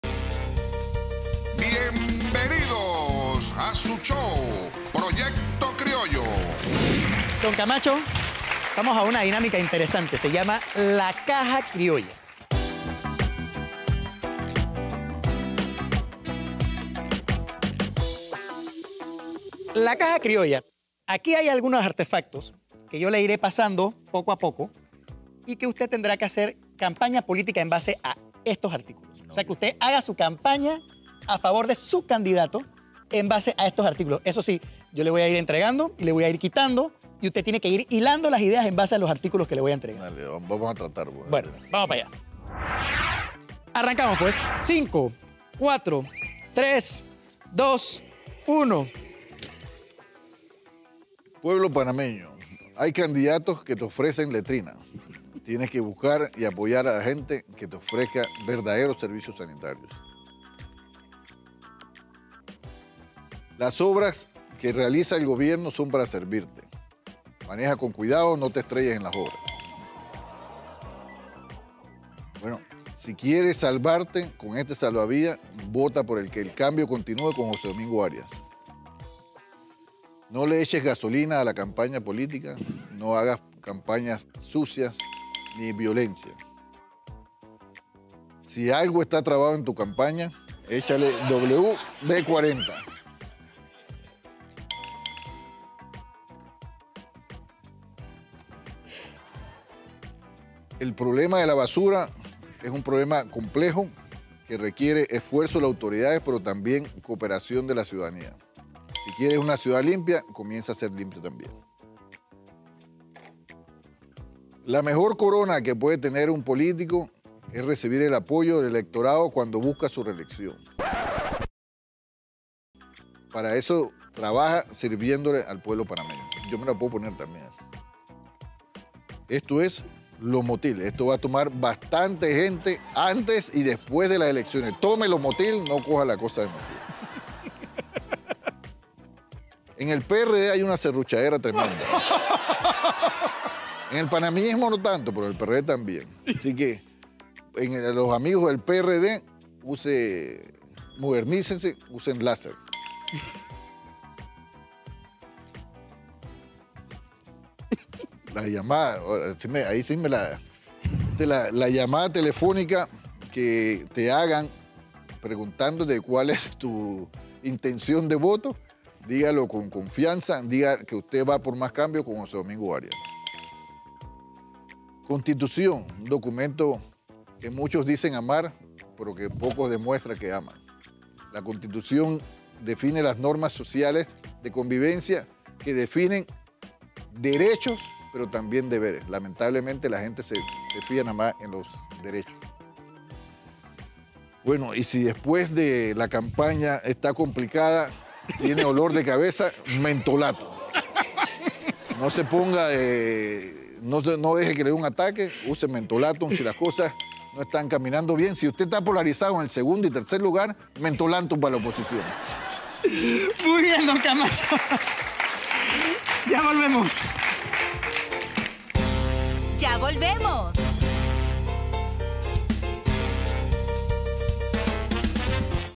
LA CAJA CRIOLLA CON LUIS EDUARDO CAMACHO
Hicimos un experimento interesante. Le dimos a Luis Eduardo Camacho los mismos objetos que utilizó Genaro López para hacer una campaña política de bajo presupuesto, con los objetos que pusimos en la caja.  Ni el invitado ni nuestro presentador Eric Martínez (@emartineztv) saben cuáles son los objetos hasta que empieza el segmento.